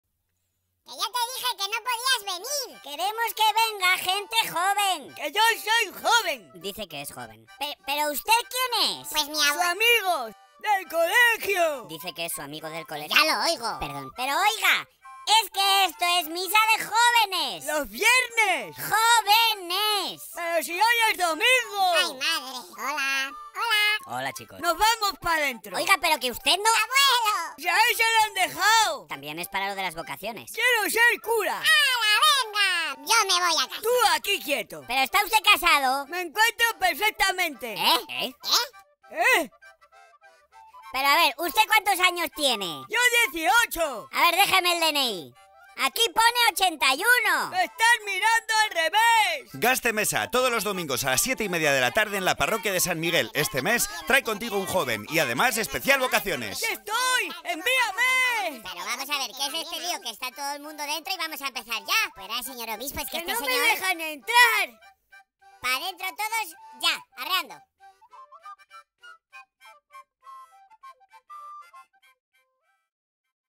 Gazte Meza - Misa Joven — Mayo 2017
Gazte Meza - Misa Joven
El mes de Mayo, Especial Vocaciones

Los domingos, a las 19:30, en la parroquia de San Miguel.
19:15, ensayo de cantos